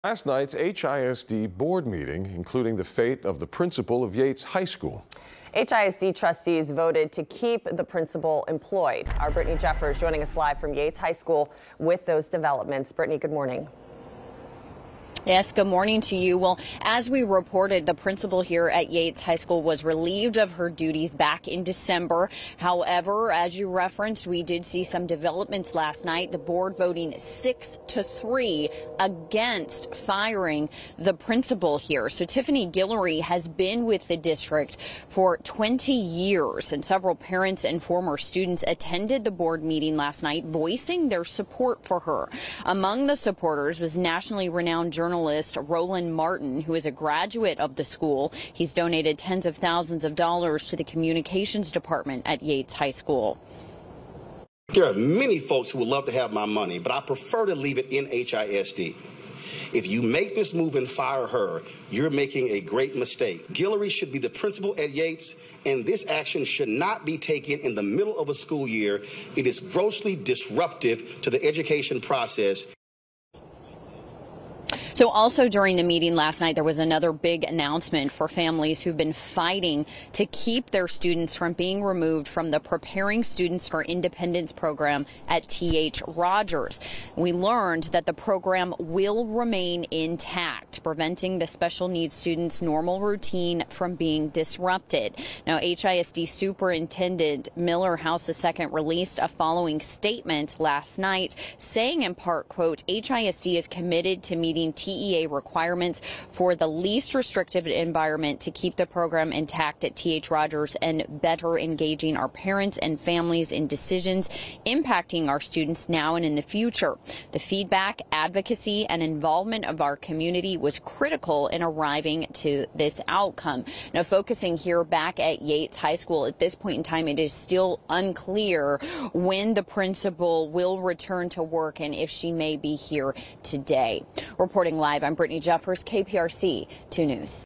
HISD board votes to keep Jack Yates principal after she was relieved of duties in December
The Houston Independent School District has voted to keep a principal who was recently relieved of her duties.